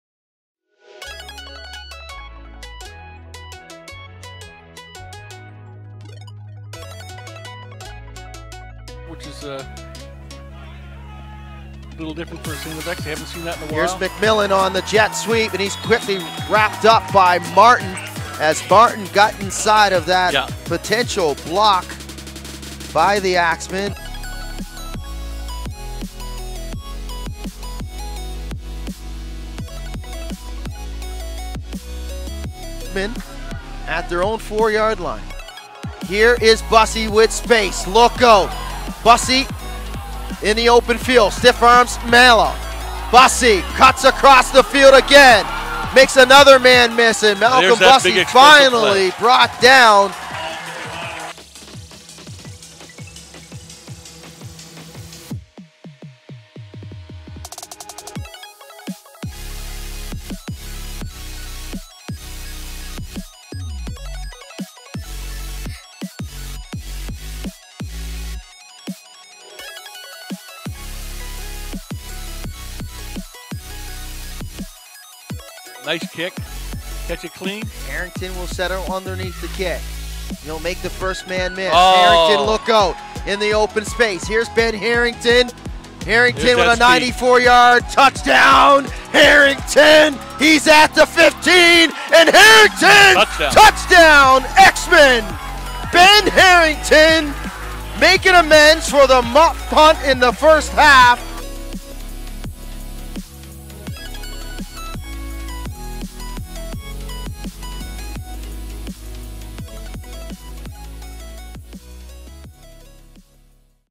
StFX X-Men Football Plays of the Week 09/03/23
StFX X-Men Football Top 3 Plays of the Week, from the week ending Sept. 3, 2023